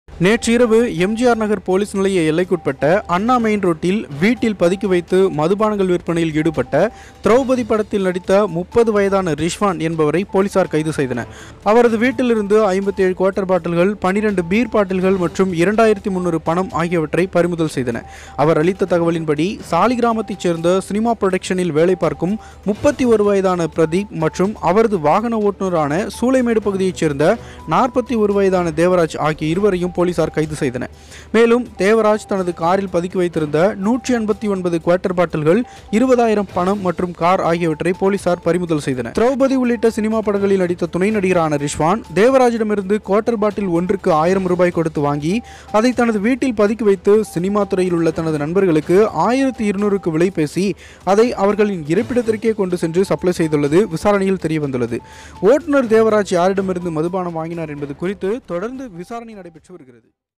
திருட்டுத்தனமாக மதுபாட்டில் விற்று மாட்டிக்கொண்ட திரௌபதி பட நடிகர்
நேற்று இரவு எம் . ஜி . ஆர் நகர் போலிஸ் நிலைய எல்லைக்கு உட்பட்ட அண்ணா மெயின் ரோட்டில் வீட்டில் பதுக்கி வைத்து மதுபானங்கள் விற்பனையில் ஈடுபட்ட திரௌபதி பட துணை நடிகர் ரிஸ்வான் ( 30 ) என்பவரை போலிசார் கைது செய்தனர் . அவரது வீட்டில் இருந்து 57 குவார்ட்டர் பாட்டில்கள் , 12 பீர் பாட்டில்கள் , ரூ . 2300 பணம் பறிமுதல் செய்யப்பட்டது . அவர் அளித்த தகவலின்படி சாலிகிராமத்தைச் சேர்ந்த சினிமா புரொடக்ஷன் வேலை பார்க்கும் பிரதீப் ( 31 ) மற்றும் அவரது வாகன ஓட்டுனரான சூளைமேடு பகுதியை சேர்ந்த தேவராஜ் ( 41 ) ஆகிய இருவரையும் போலீசார் கைது செய்தனர் . மேலும் தேவராஜ் காரில் பதுக்கி வைத்து இருந்த 189 குவார்ட்டர் பாட்டில்கள் , 20 , 000 பணம் மற்றும் கார் பறிமுதல் செய்யப்பட்டது . திரௌபதி உள்ளிட்ட சினிமா படங்களில் நடித்த துணை நடிகரான ரிஸ்வான் , தேவராஜிடம் இருந்து குவார்ட்டர் பாட்டில் ஒன்று ரூ . 1000 கொடுத்து வாங்கி அதை வீட்டில் பதுக்கி வைத்து சினிமா துறையில் உள்ள தனது நண்பர்களுக்கு ரூ . 1200க்கு விலை பேசி அதை அவர்களின் இருப்பிடத்திற்கே கொண்டு சென்று சப்ளை செய்து வந்துள்ளது விசாரணையில் தெரியவந்துள்ளது . ஓட்டுநர் தேவராஜ் யாரிடம் இருந்து மதுபானம் வாங்கினார் என்பது குறித்து தொடர்ந்து தீவிர விசாரணை நடைபெற்று வருகிறது